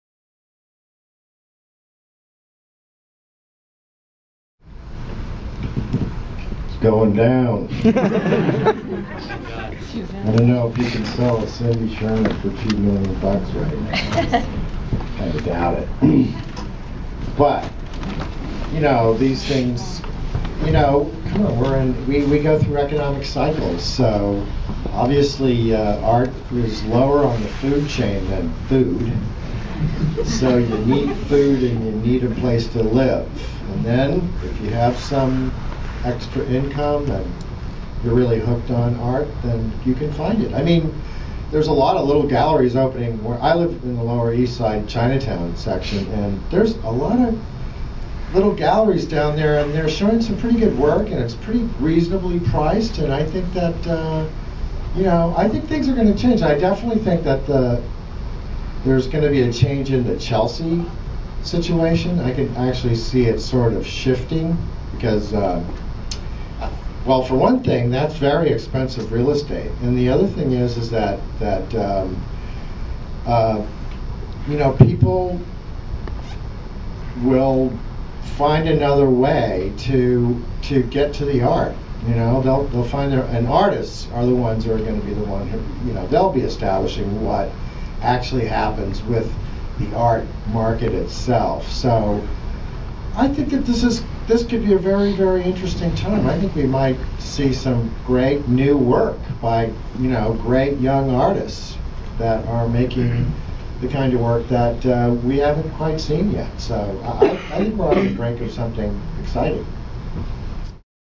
Paul H-O talks about whether these are Hard Times for artists. video clip by Susan Shaw
Artist and TV personality Paul Hasegawa-Overacker takes questions after a screening of his new film,  "Guest of Cindy Sherman." In this clip he discusses the recent hard times and its effect on artists.